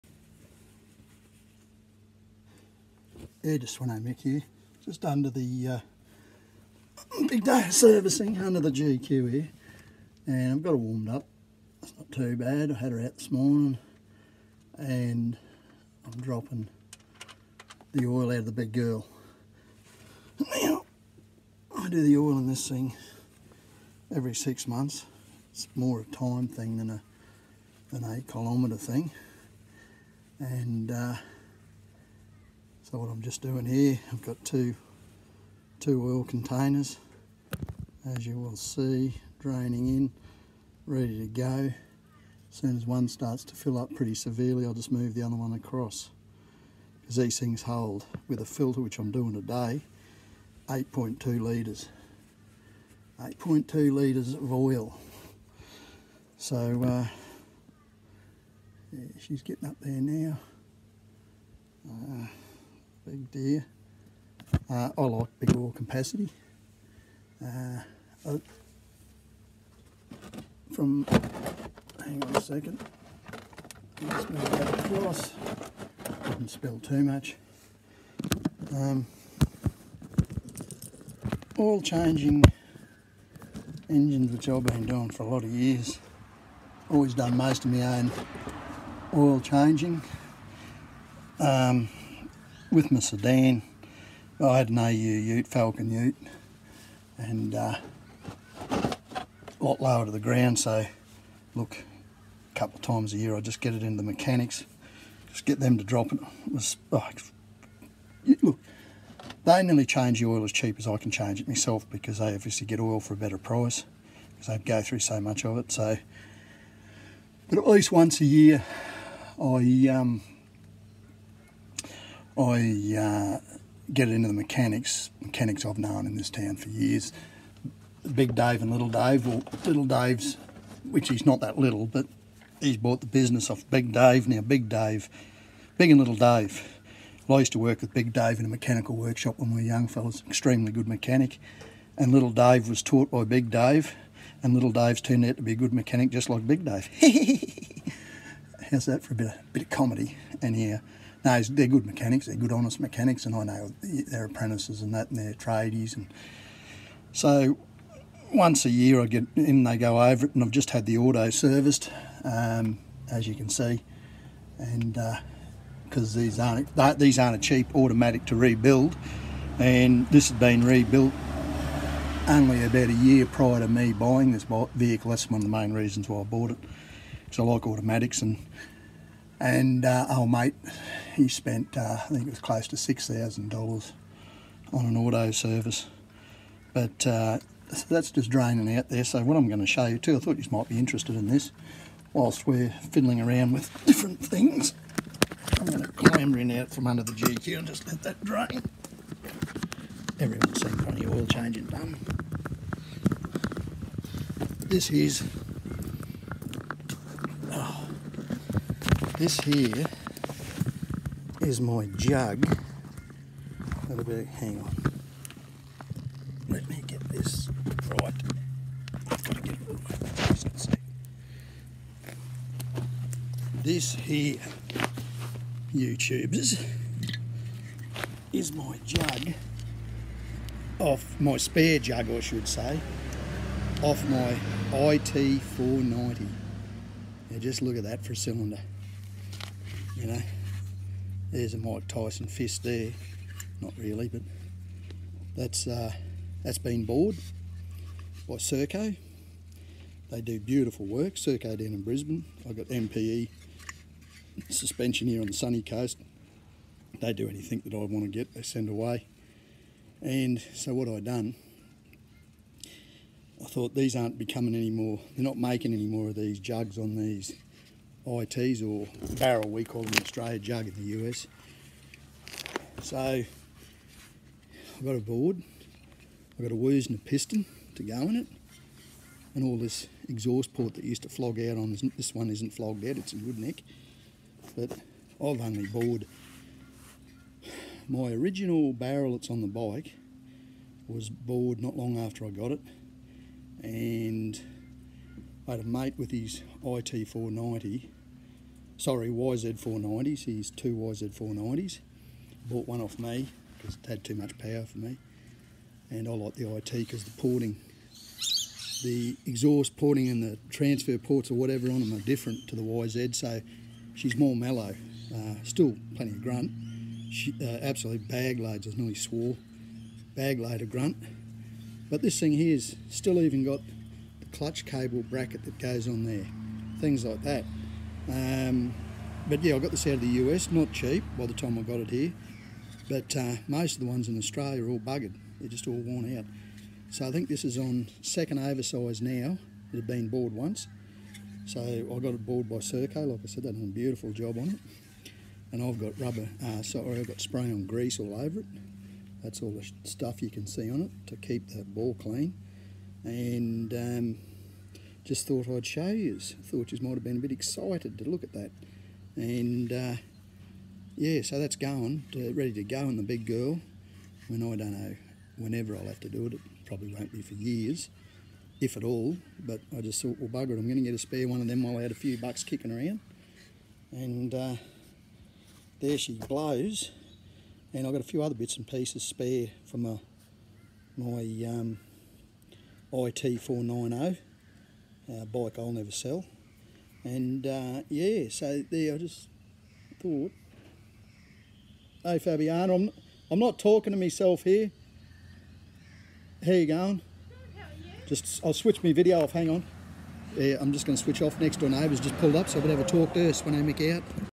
Oil change the GQ TB 42 Patrol and Yamaha IT 490 Barrel Maintenance Day
Patrol TB 42. Yamaha IT 490. Sveno mick. Maleny. QLD.